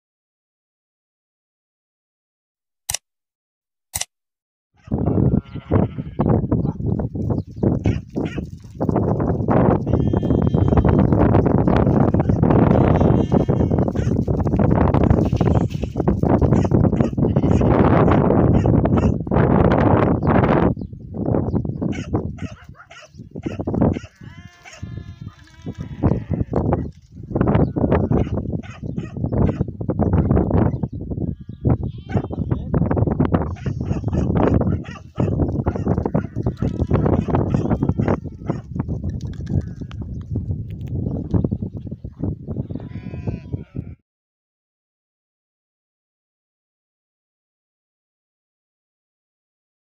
طليان حريات عدد ١٠روس للبيع التفاصيل☜رقم راعيها ٠٥٤١٠٠٤٨٧٠
حراج على طليان حريات عدد ١٠روس
٨طليان ٢رخال العمر ٤شهور وفوق
الموقع /   جده الخمره
رقم الجوال/   ٠٥٤١٠٠٤٨٧٠